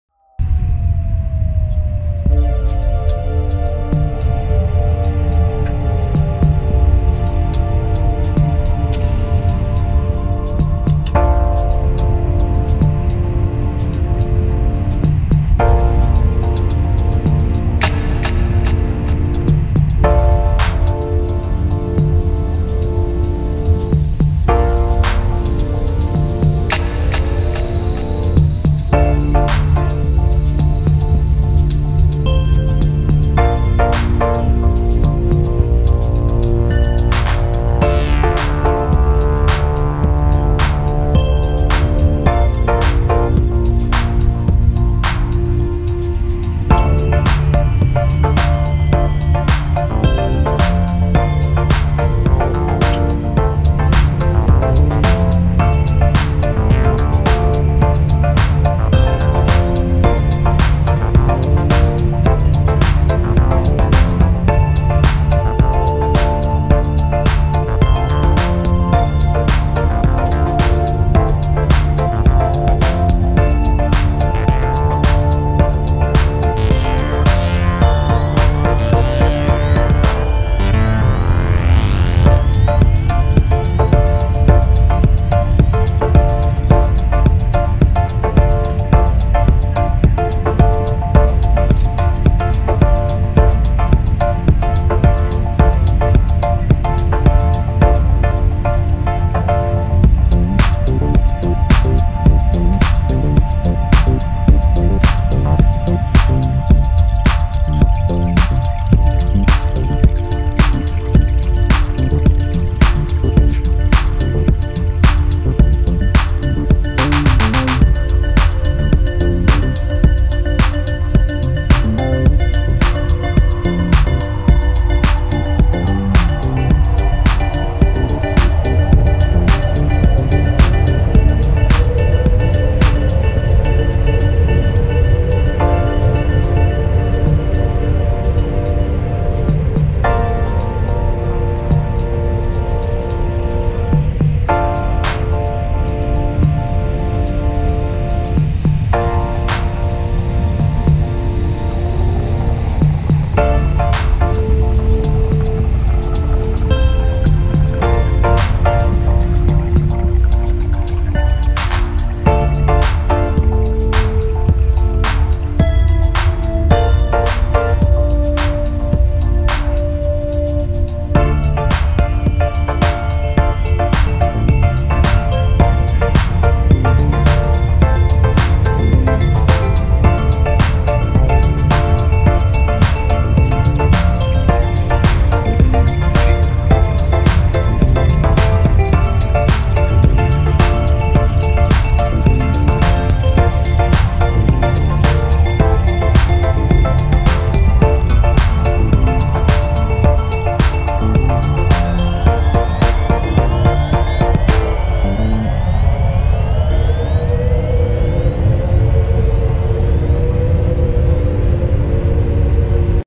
Korean fast train (KTX) Busan to Daegu (08.2019)
Short trip with Korean fast train (KTX - Korean Train eXpress) from Busan to Daegu. Maximal speed 305 km/h. Average speed 250 km/h.